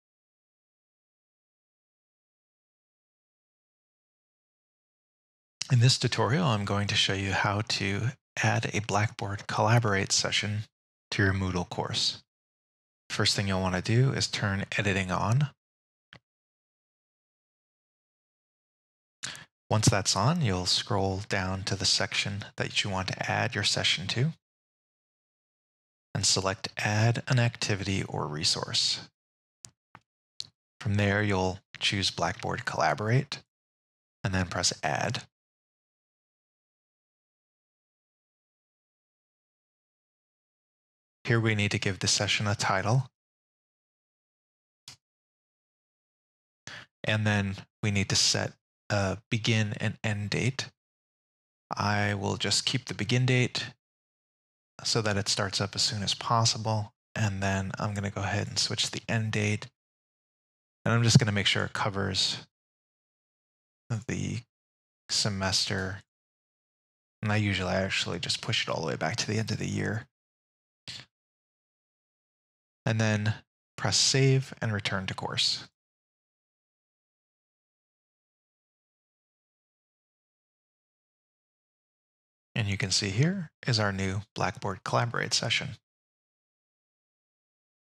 Blackboard Collaborate - Add a Session to a Moodle Course
This tutorial shows you how to add a Blackboard Collaborate session to your Moodle course.